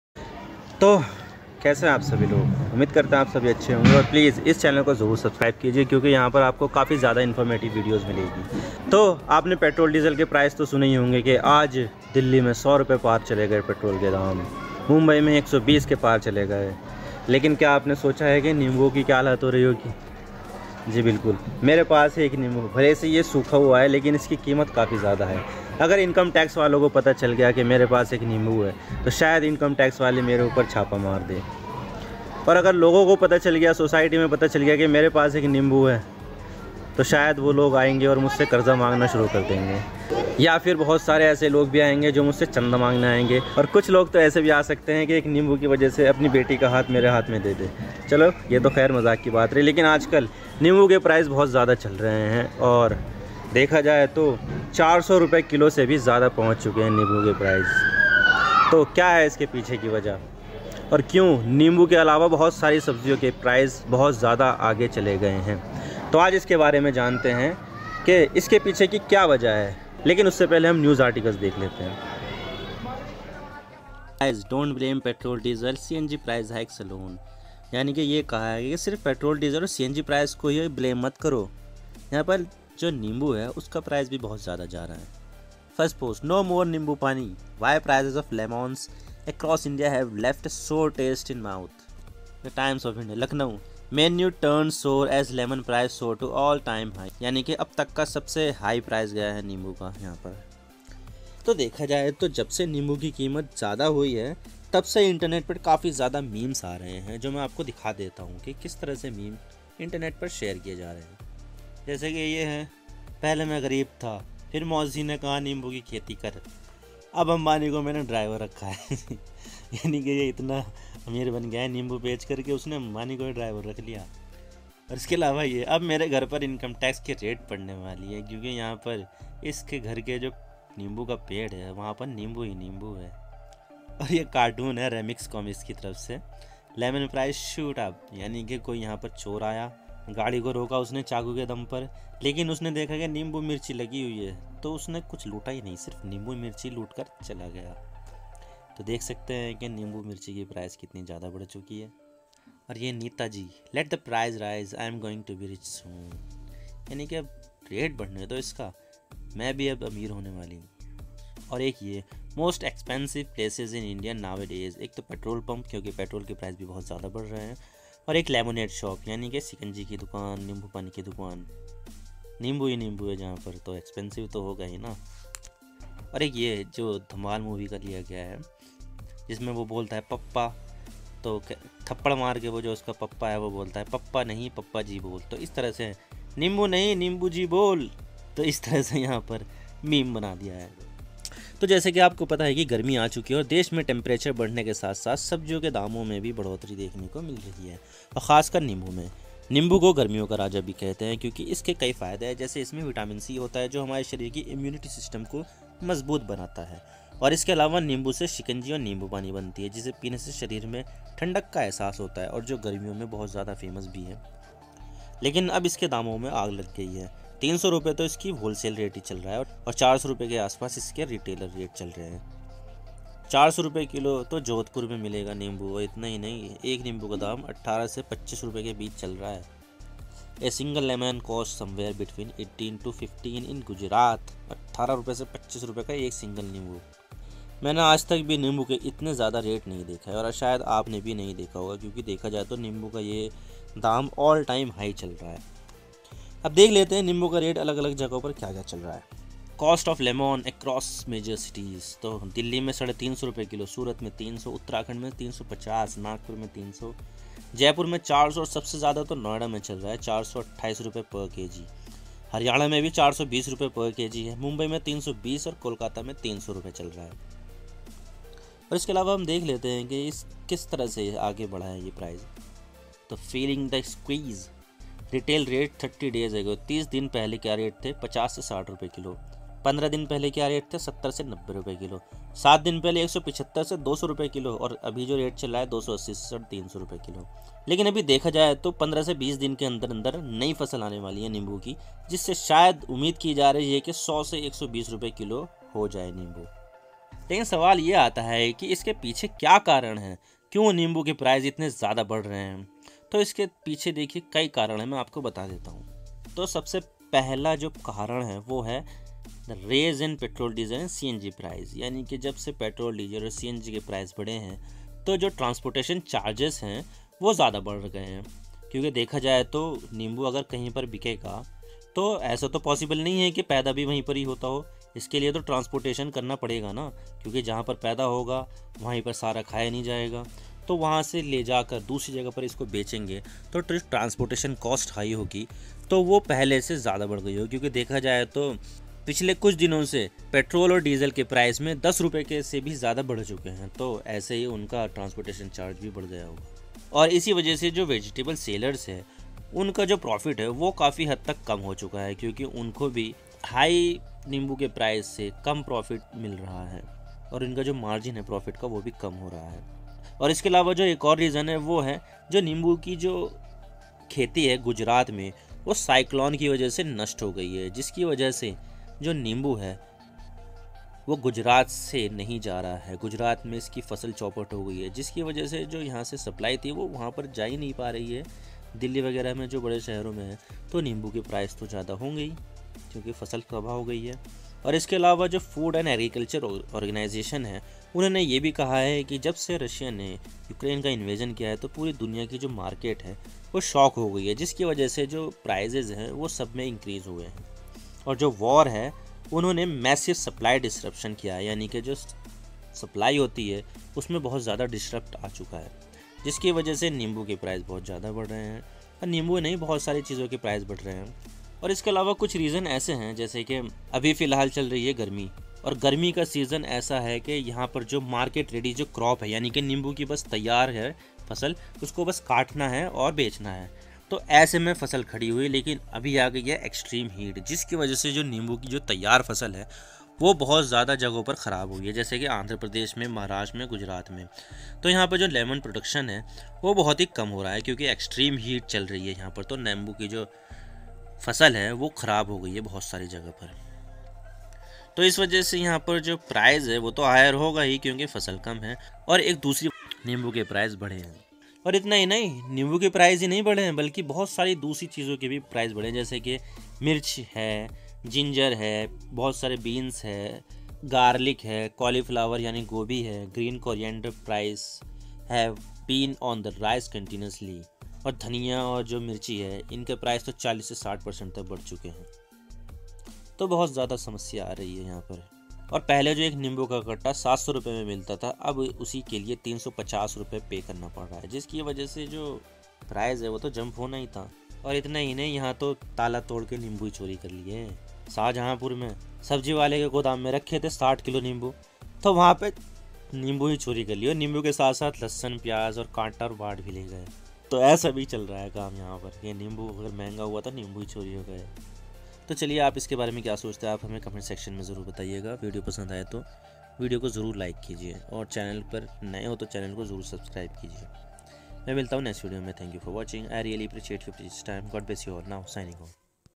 Why Lemon Price Become All Time High? | Reason Behind This? नींबू क्यों इतना महंगा हुआ? | Lemons
Hi Friends, In this video We will talk about why lemon price getting high in recent days? what are the reasons behind it? lemon memes? let's see

Host: Imran Siddiqui
Voice: Imran Siddiqui
Edited By: Imran Siddiqui

All Advice, Suggestion, Criticism and Appreciation Are Welcome